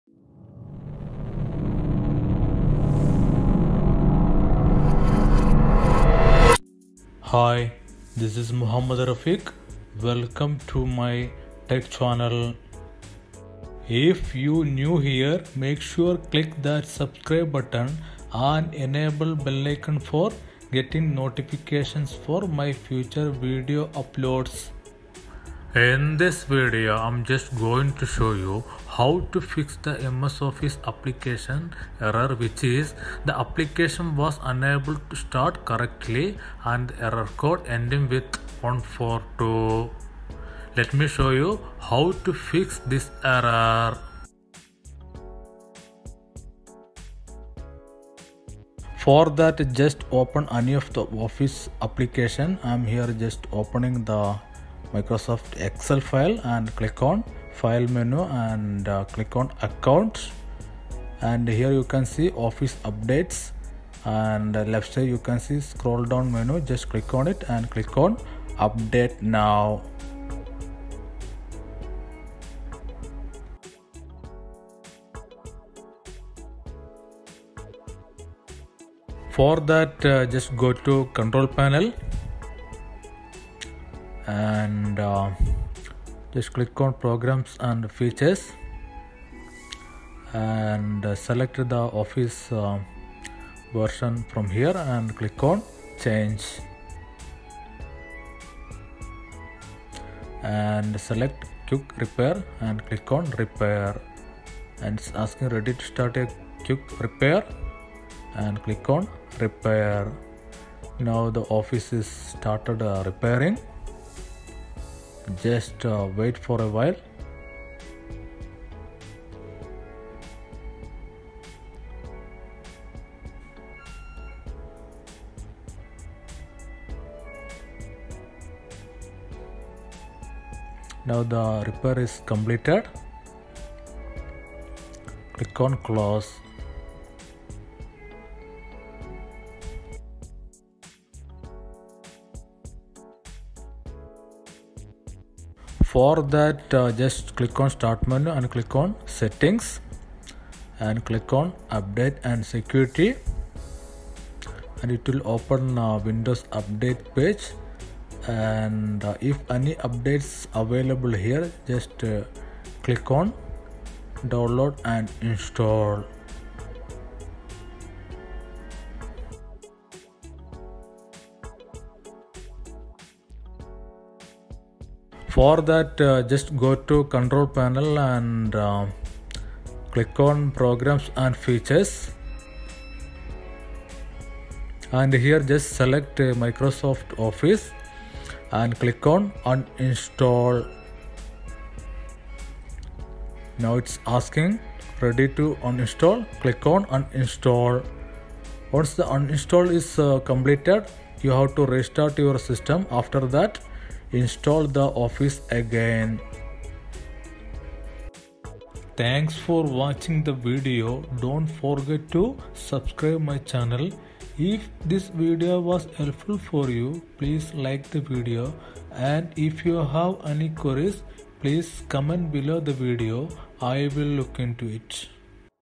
How to fix Microsoft Office Application Error 0xC0000142
This video is a tutorial for How to fix Microsoft Office Application Error 0xC0000142

Error details:
Microsoft Office Application Error 0xC0000142
The application was unable to start correctly (0xC0000142)

Reason:
There are many reasons behind this error. Some are Office Updates, Office Corruption, Windows Updates etc.

Solution:
There are many troubleshooting methods to solve this Microsoft Office Application error.
Method 1: Update Microsoft Office
Method 2: Repair Microsoft Office
Method 3: Run Windows Updates
Method 4: Uninstall & Reinstall Microsoft Office